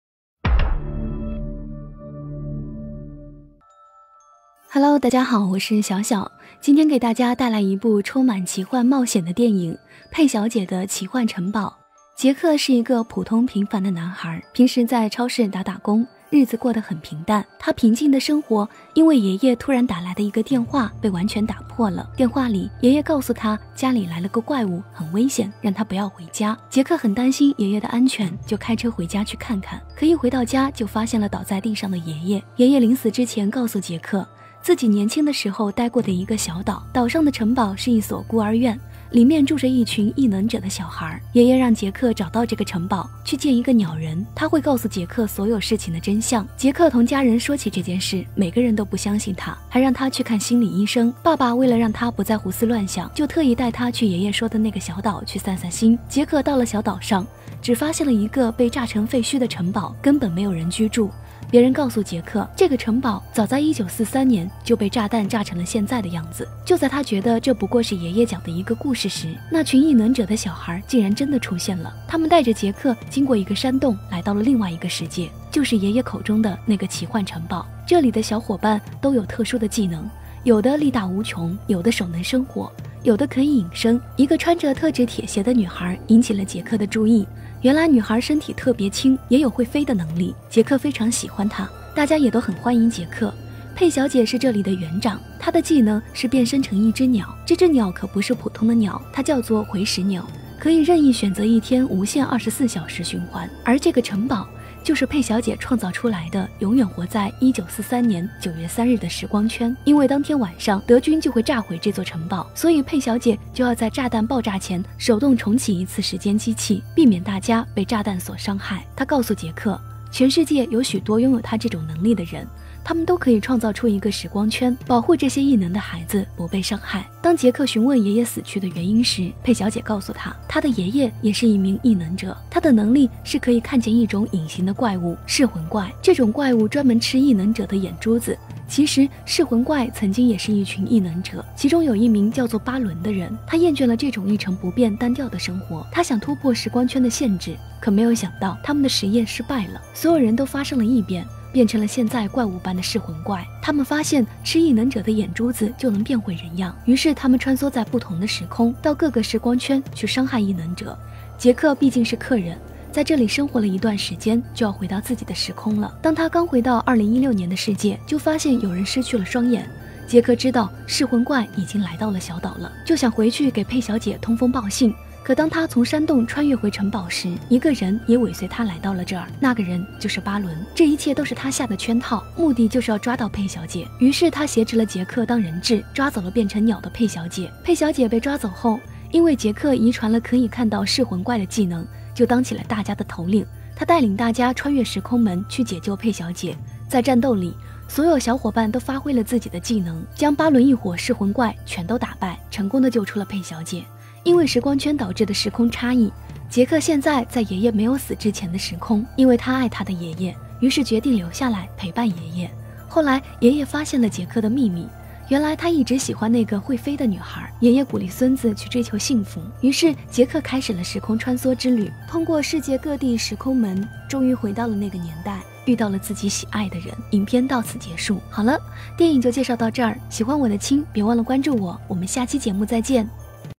小姐姐讲电影：小女孩身体比羽毛轻，每天必须穿上30公斤的铁鞋，不然就会飞上天
讲电影官方，明明更新